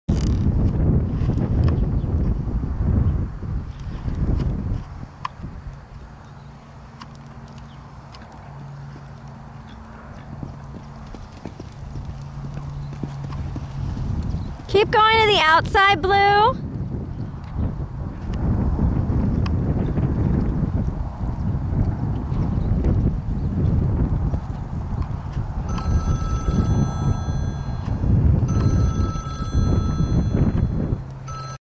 Allie Med pony for sale Crossrail Line
For sale 13.1 med pony mare
8 years old. Still eligible Green and
has plenty of local show miles. Allie is a Short Sturrips
packer and is ready move up to the med ponies or do Childrens pony.
She is the sweetest mare, very kind, easy to handle, clips, loads,
Trial rides and loves attention!! Allie is a good mover/jump and VERY comfy!
 She goes in an Eggbut snaffle on a soft loopy rein.
He changes are PERFECT every time, no matter who is asking!